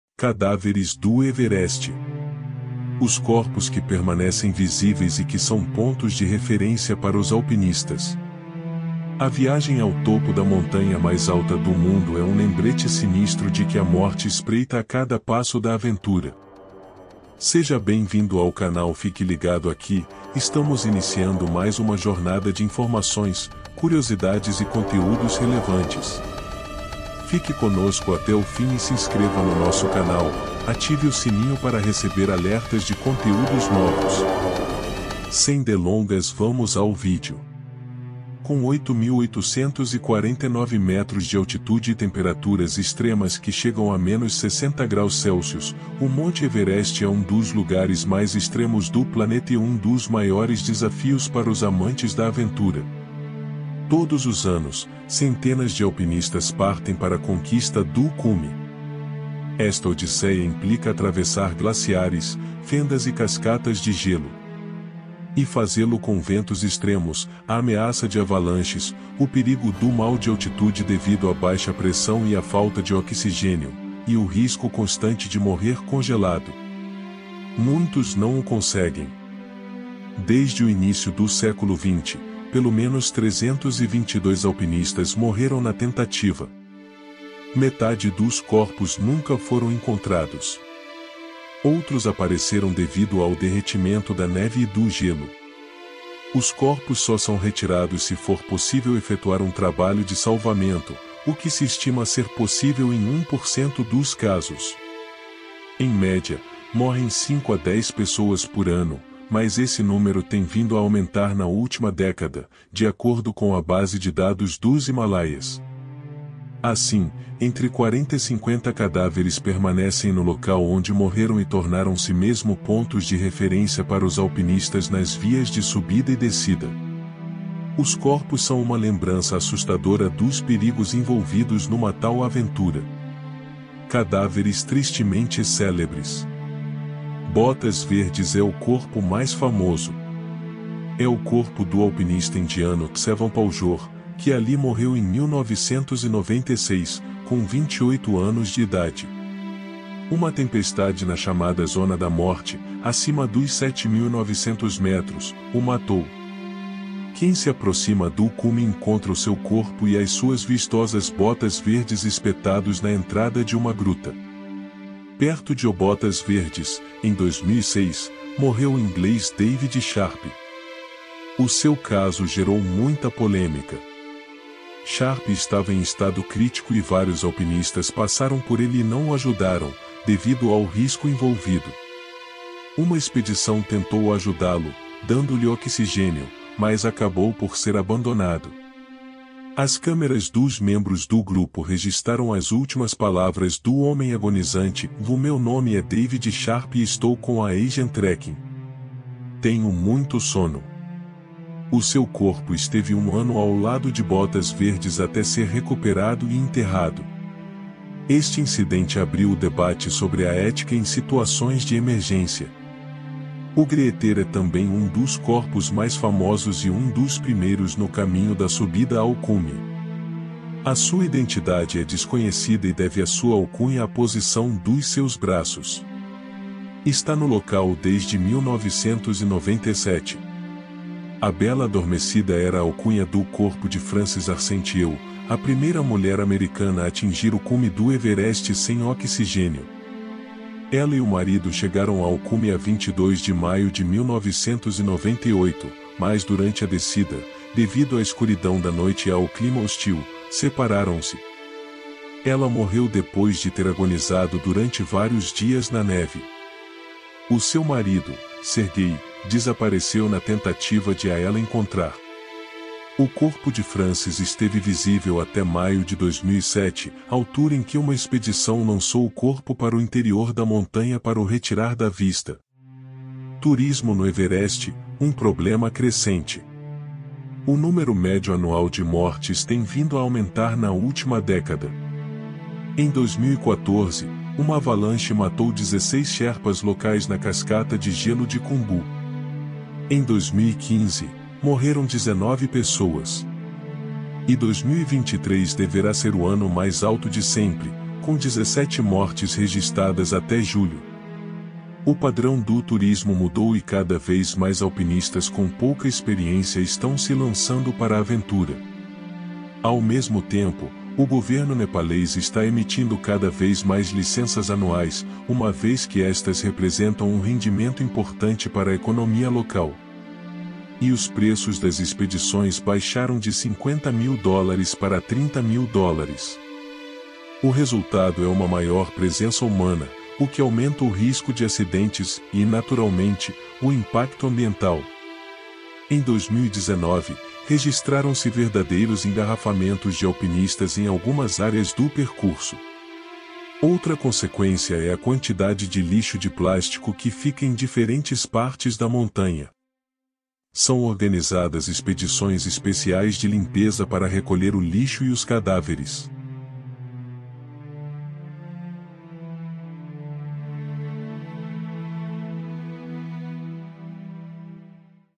Inacreditável: cadáveres do Monte Everest encontrados no gelo!
Neste vídeo, testemunhe a inacreditável descoberta de cadáveres no Monte Everest encontrados congelados no gelo. Junte-se a nós enquanto exploramos o encontro chocante e revelamos a terrível verdade por trás dessa descoberta arrepiante.